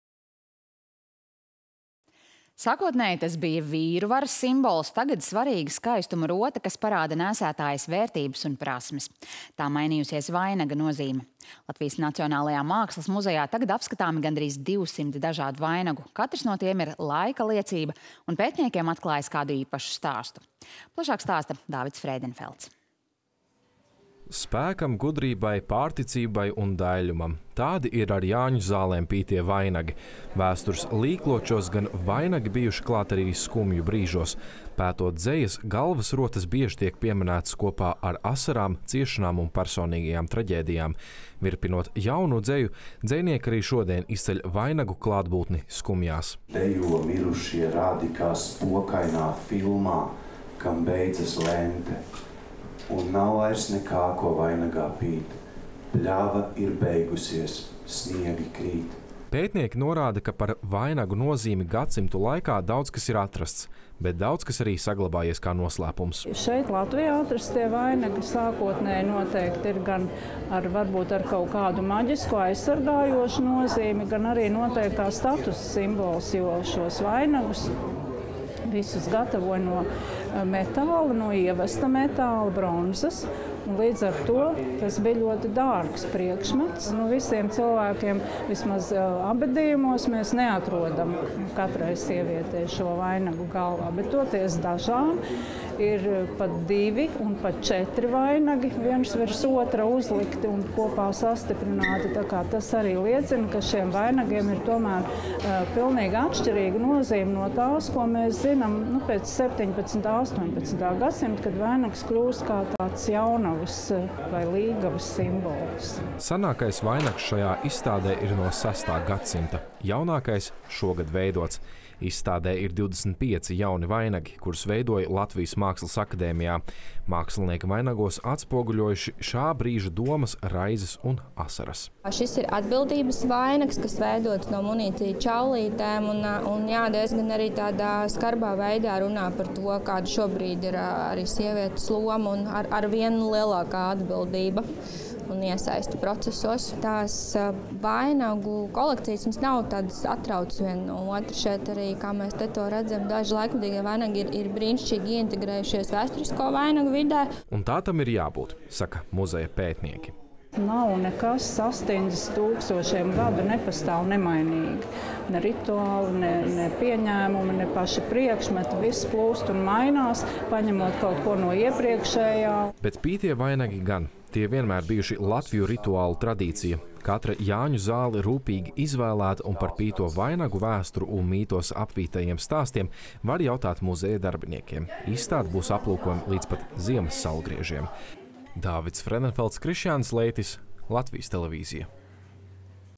Vainagu vēsture – līkloču pilna
Sākotnēji tas bija vīru varas simbols, tagad – svarīga skaistuma rota, kas parāda nēsātājas vērtības un prasmes. Tā mainījušās vainaga nozīmes. Latvijas Nacionālajā vēstures muzejā tagad apskatāmi gandrīz divsimt dažādu vainagu – katrs no tiem ir laika liecība un pētniekiem atklājis kādu stāstu. Plašāk stāsta Dāvids Freidenfelds.